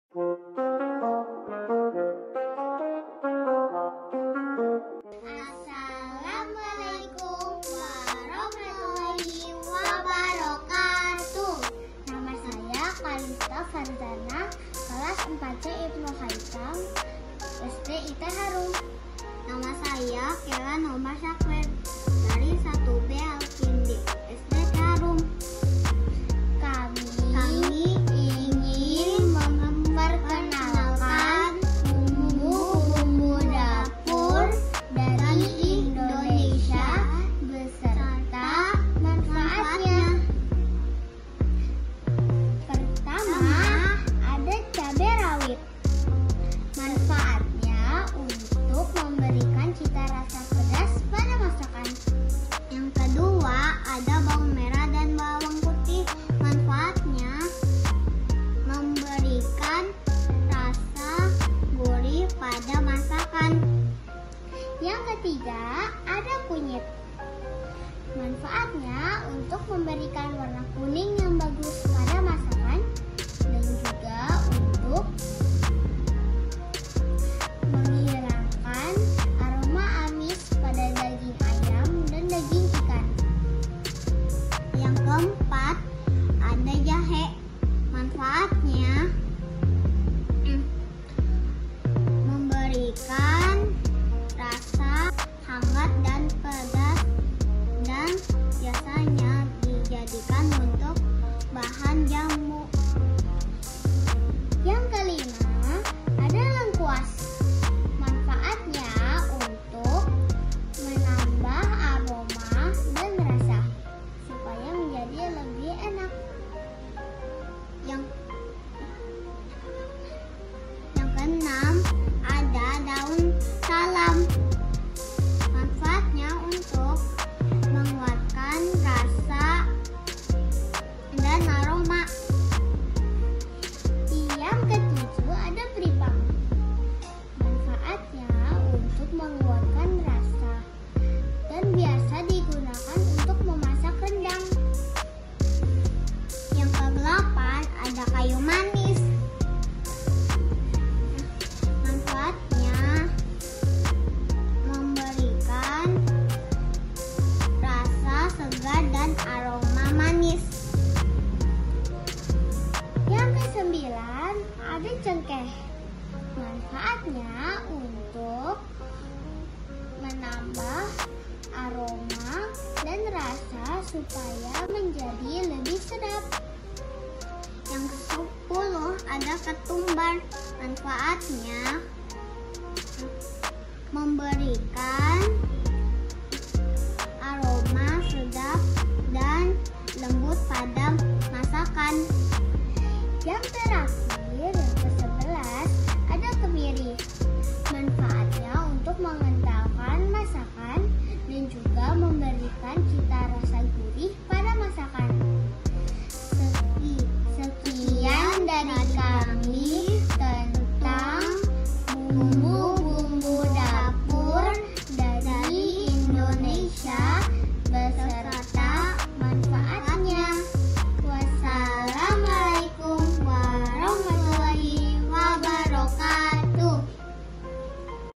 Giat Pramuka SIT Harum. Callysta Farzana Kelas 4C Ibnu Khaitam Kellan Omar Shaquell 1B Al Kindi SDIT
Kegiatan Pramuka SIT Harum.
Callysta Farzana Kelas 4C Ibnu Khaitam
Kellan Omar Shaquell 1B Al Kindi
SDIT Harum Jakarta Utara